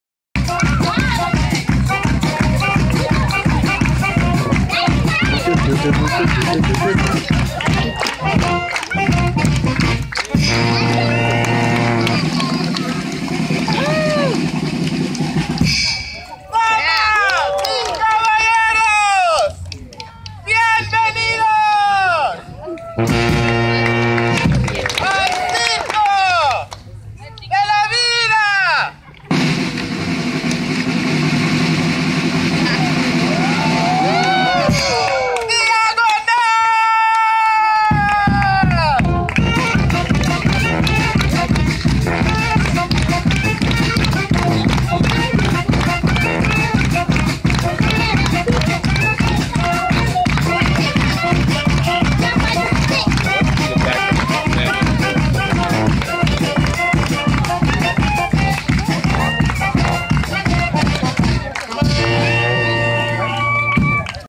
Introductory song & Dance - Bread & Puppet
Here is the introductory song and dance of the Bread & Puppet Theater presentation at the Bread & Roses Festival in Lawrence Massachusetts on Labor Day Sep 2 2019.